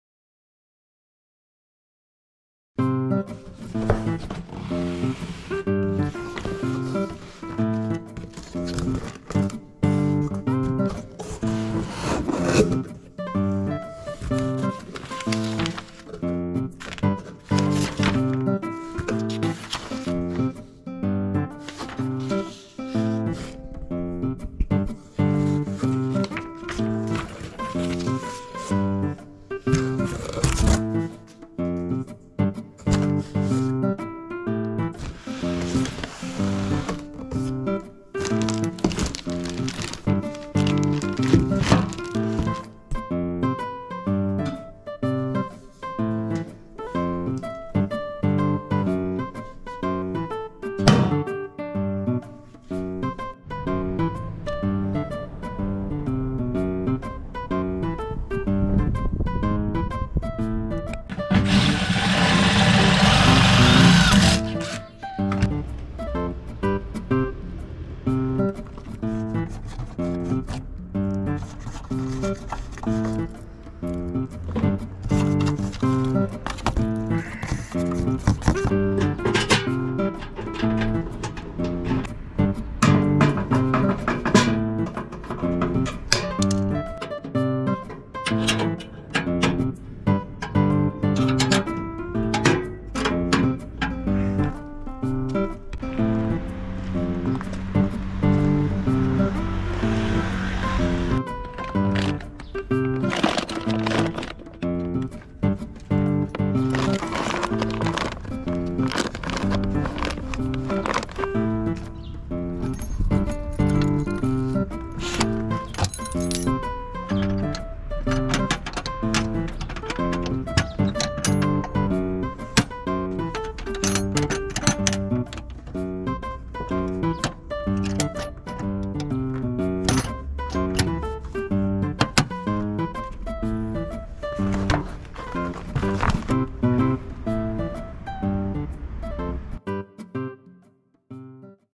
ハイゼットテールリフト 工具箱 軽トラ
ハイゼットテールリフトに工具箱を取付しました。